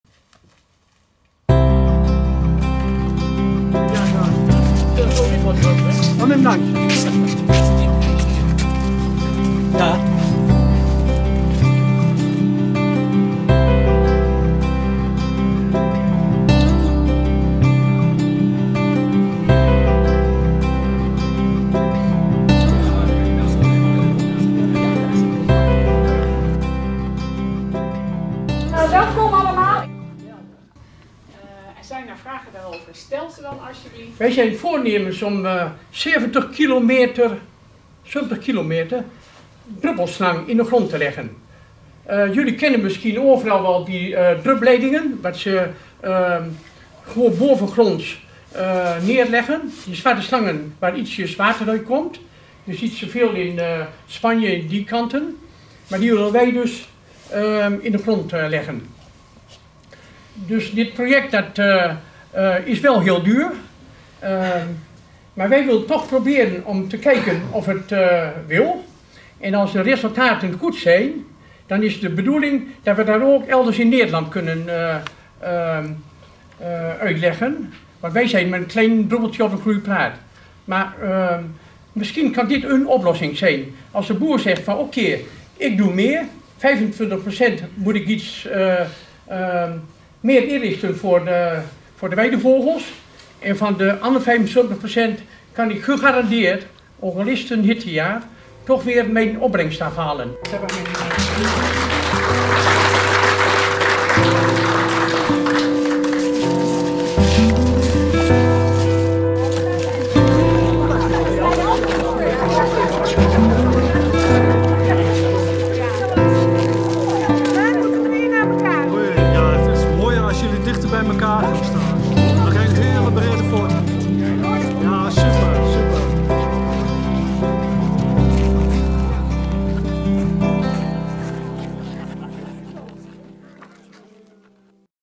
Innovatief project weilandirrigatie warm onthaald
Zo'n 70 kilometer druppelslang legt de Vogelwerkgroep Geesteren samen met boer Chiel Kamphuis dit jaar zo'n 10 cm diep in het grasland net buiten het dorp. Het gaat om een uniek project. dat nu steun krijgt van SBNL Natuurfonds.

Kruidenrijk
Een kwart van dit land van boer Chiel is straks weidevogelgebied in het voorjaar. De kuikens vinden er in het kruidenrijke weiland dekking en voedsel. De overige 75% van het land kan de boer maximaal benutten voor zijn vee.

Droogte
De druppelirrigatie zorgt voor een optimale opbrengst omdat er altijd voldoende water is. Droogte en klimaatverandering hebben geen invloed meer op de groei van het gras. Boer en weidevogel profiteren van het nieuwe project.

Toekomst
Voorzitter Hennie Schröder van de Vogelwerkgroep Geesteren legt in de video uit dat het project in de toekomst misschien uitgerold kan worden in een groot deel van Nederland.